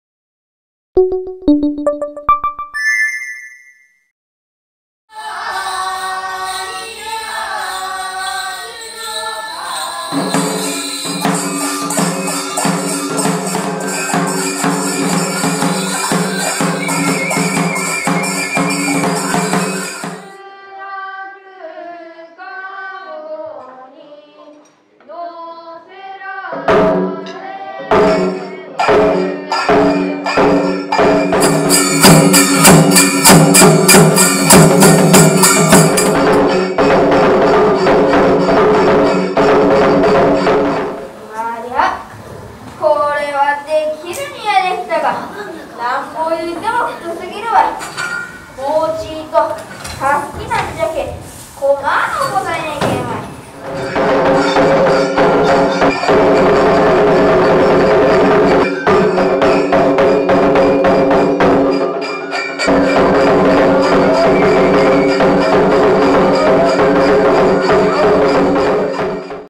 【いまできること】大草神楽子ども研究クラブ
今年、設立40年を迎える大草神楽子ども研究クラブ。記念行事で神楽を披露するために、メンバーは練習に励んでいた。しかし、7月6日の西日本豪雨により、神楽の道具を保管していた倉庫は流され、練習場所の大草公民館は床上浸水。長く大切に使ってきた、高価な衣装や道具が泥に浸かり、使えなくなってしまった。
「今年の公演は無理か」という雰囲気の中、メンバーとその家族、関係者たちは「地域の伝統芸能を絶やしたくない」と奮闘。募金活動やさまざまな支援により、9月に何とか最低限の道具を揃え、神楽公演をスタートさせた。

取材・撮影：ぶるぼん企画室 堀行丈治・門田聖子

「いまできること」 平成30年7月豪雨 WEBサイト